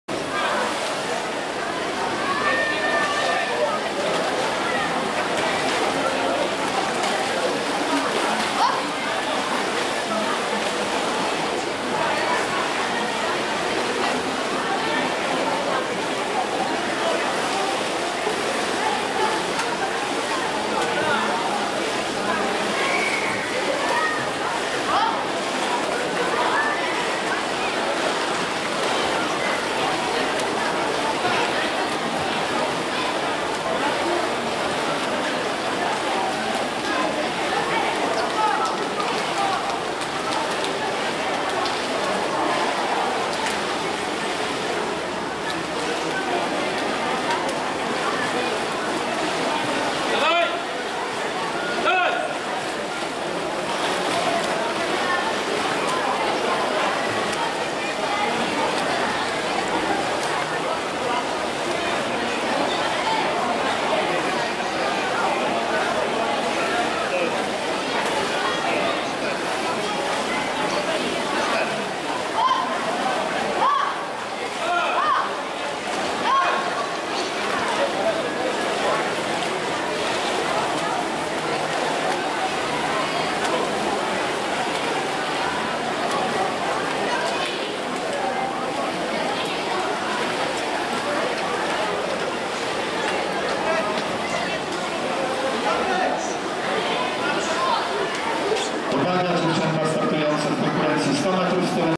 K. Tkaczuk (4) i J. Wesołowski (5), 100 m klasycznym
VII Międzynarodowy Mecz Pływacki Dziecięcych Drużyn Klubowych z Litwy, Białorusi, Ukrainy i Polski "Złota Jesień 2013", Bielsk Podlaski, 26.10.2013